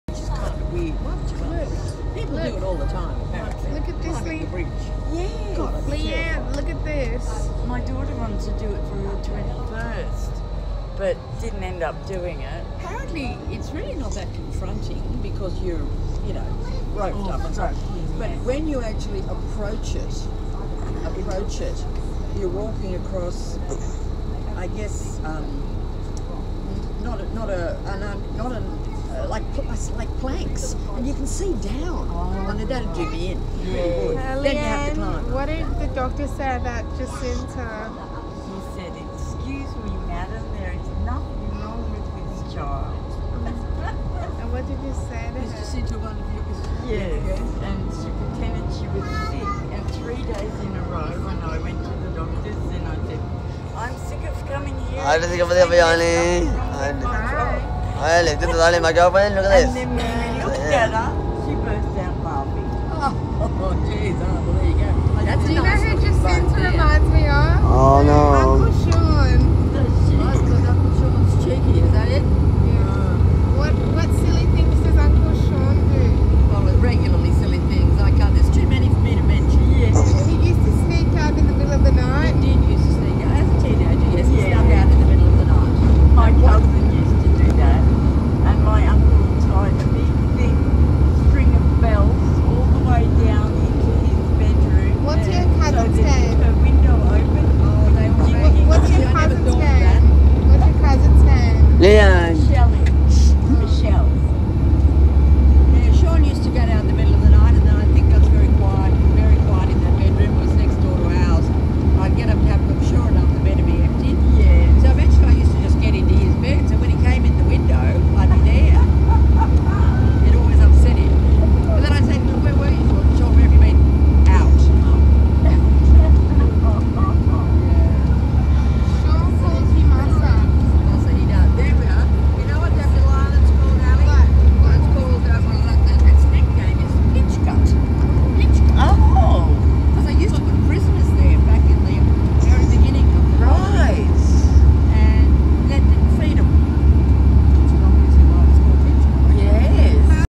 Sydney Ferries Pemulwuy Emerald Class Ferry Pemulwuy Route F9
We are going to Watson’s bay and I’m in the ferry now with my mum and my girlfriend and her grandma